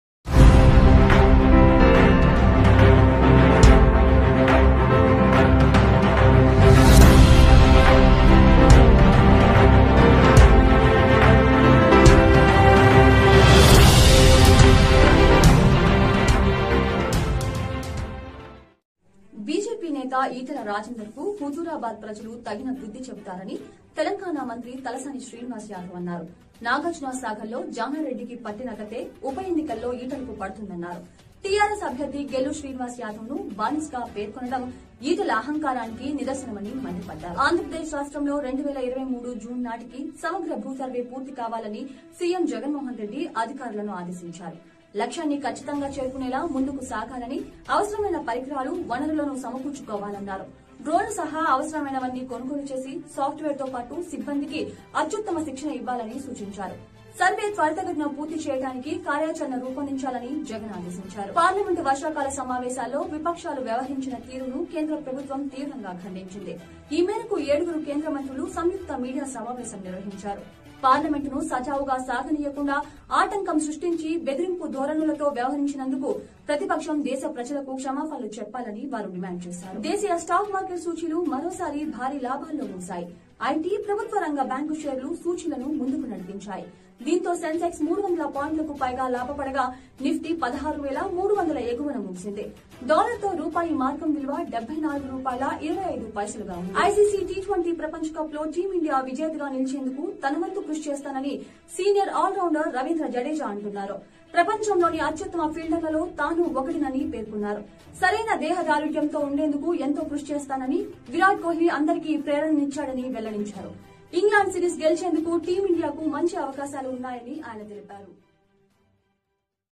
Top 5 Latest News: Talasani Srinivas Yadav Fires on Etela Rajender over his Comments|| TTT NEWS
Top 5 Latest News: Talasani Srinivas Yadav Fires on Etela Rajender over his Comments on Gellu Srinivas || Ttt news